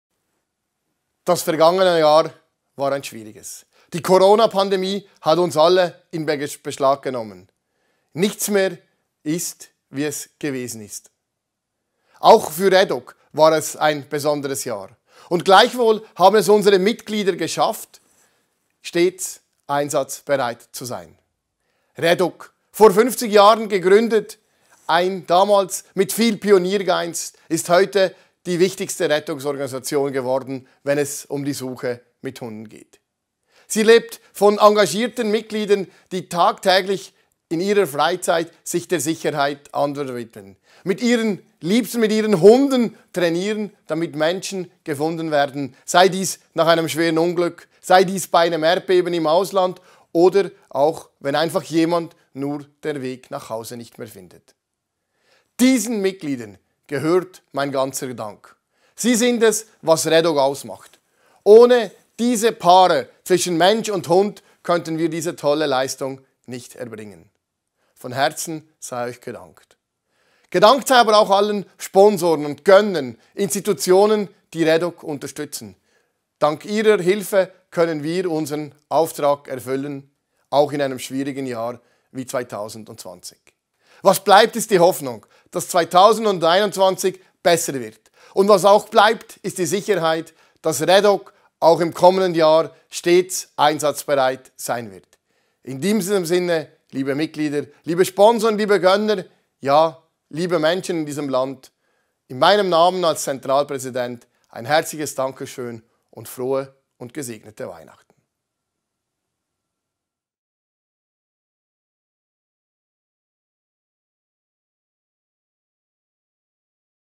REDOG Messagio di Natale 2020
Philipp Matthias Bregy, Presidente centrale di REDOG, guarda al 2020 e augura buone feste.